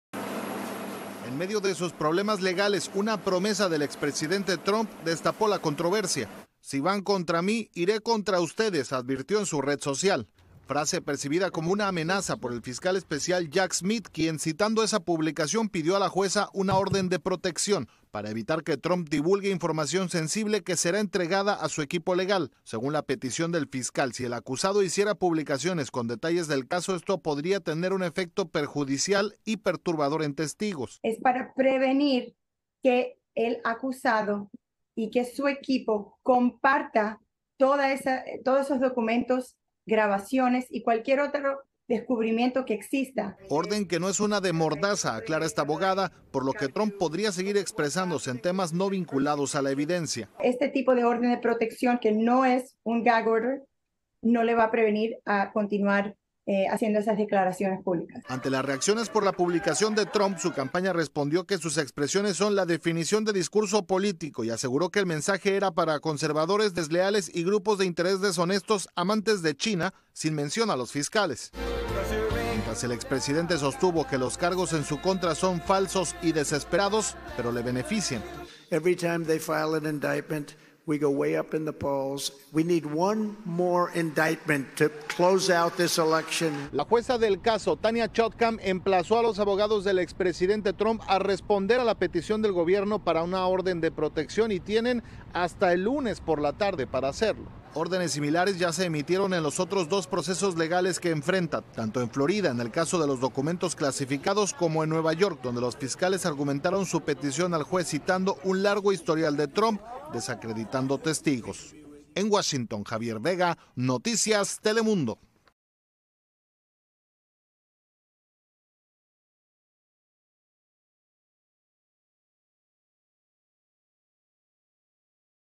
Piden a jueza de caso contra Trump que imponga reglas | Noticias Telemundo
Video oficial de Noticias Telemundo. "Si van contra mí, iré contra ustedes", publicó el expresidente en su red social. Por eso el fiscal de caso solicitó una orden de protección, mientras el Departamento de Justicia pedía a la jueza contener la posible divulgación de detalles del proceso.